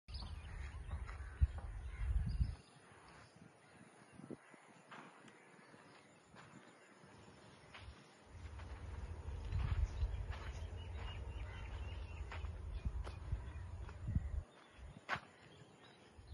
GP Realty's Woodbridge Garden - Affordable Gated Community Farmlands located at Koovathur, ECR!
Choose your finest farmhouse, Choose GP Realty! Farmhouse construction completed for our client at Woodbridge Garden! Make nature your home at Woodbridge!
Invest in your Dream Farmland at GP Realty projects located in one of the fastest growing areas. At @gprealtyindia we believe, Nature is Home!
With each farm land purchase, the following facilities are provided at Free of cost
 🌿Fencing with gate
 🌿Plantation of your choice
 🌿Drip irrigation